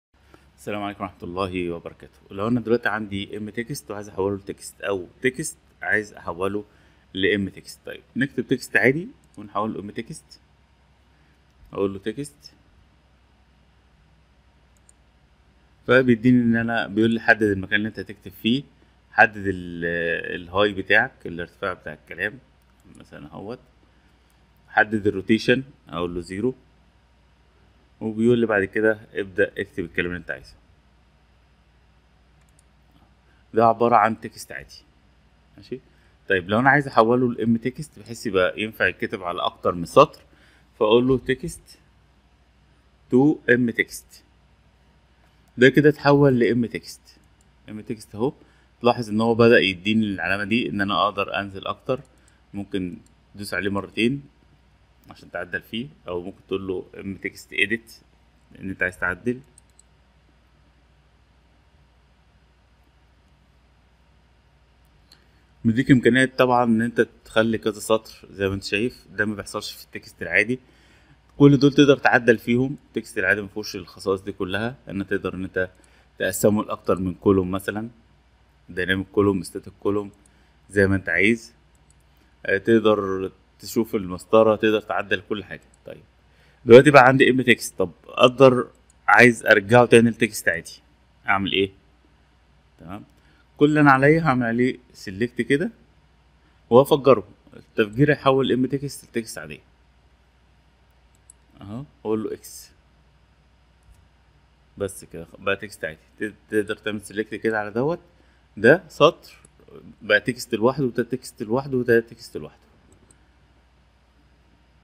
برنامج تعليمي AutoCAD تحويل Mtext إلى نص أو نص إلى Mtext AutoCAD 2015
This video will guide you how to Convert Mtext to Text or Text to Mtext AutoCAD

❤️ روابط التواصل
✅ انستجرام